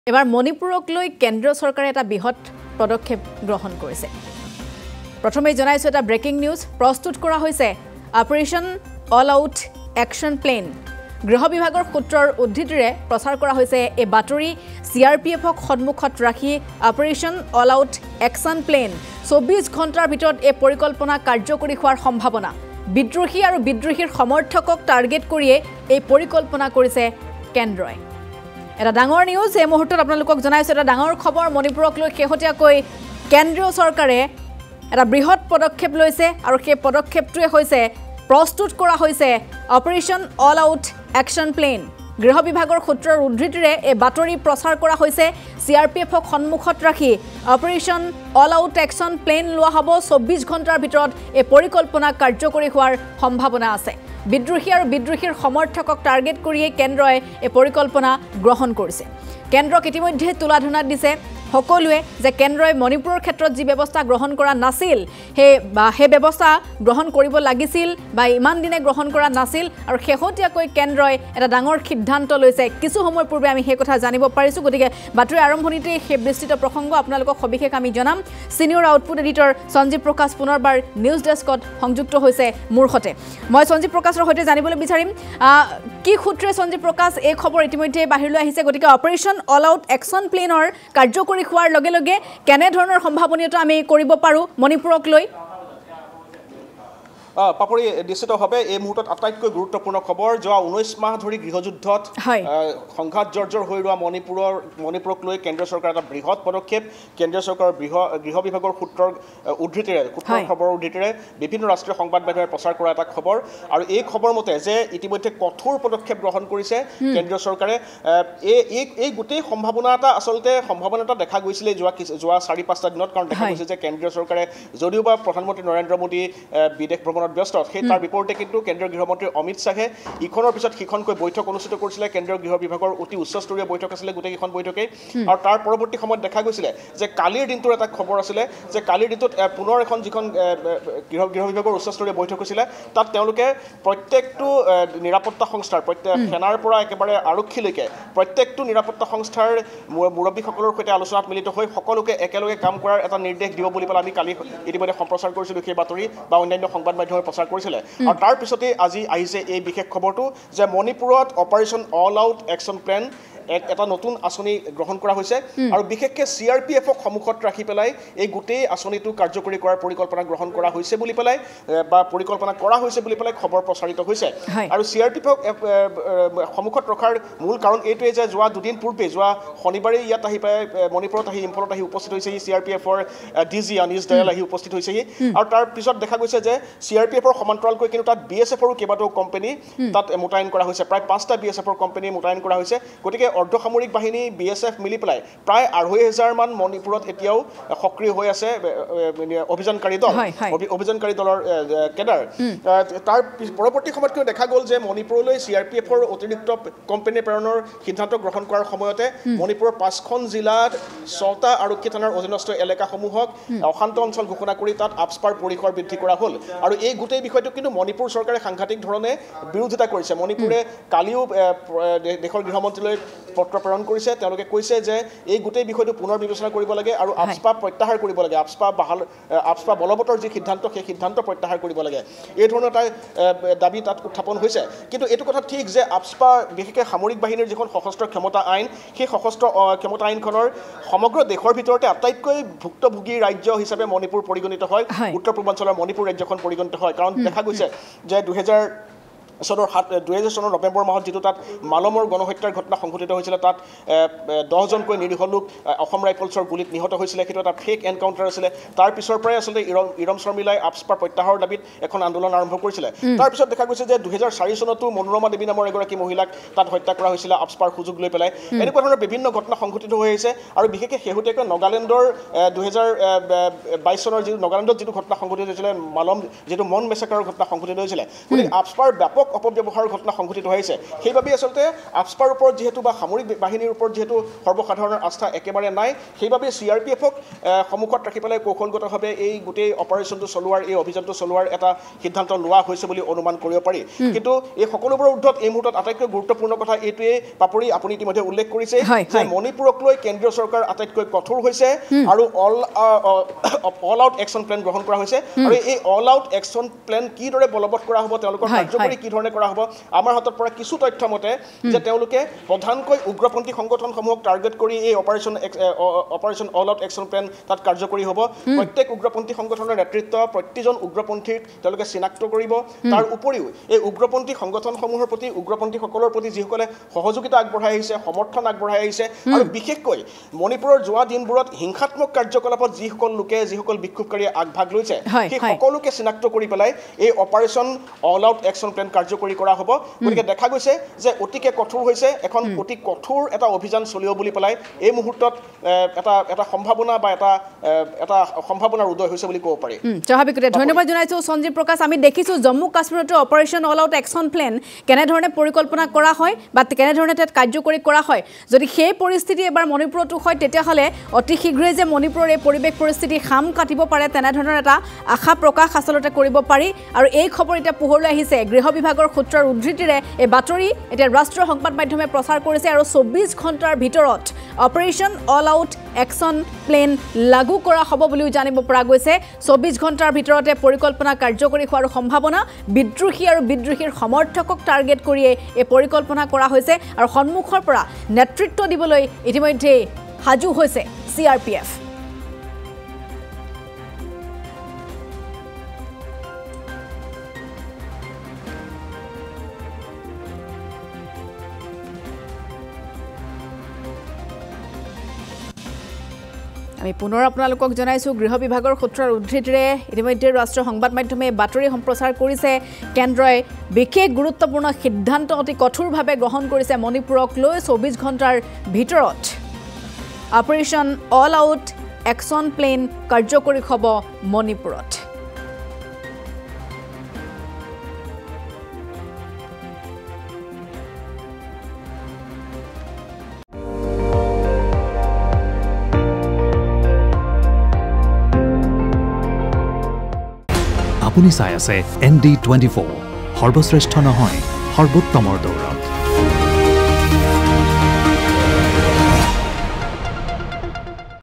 Manipur Violence: মণিপুৰক লৈ কেন্দ্ৰীয় চৰকাৰৰ বৃহৎ পদক্ষেপ
মণিপুৰক লৈ কেন্দ্ৰীয় চৰকাৰৰ বৃহৎ পদক্ষেপ৷ প্ৰস্তুত কৰা হৈছে OPERATION ALL OUT ACTION PLAN ৷ গৃহ বিভাগৰ সূত্ৰৰ উদ্ধৃতিৰে প্ৰচাৰ হৈছে এই বাতৰি৷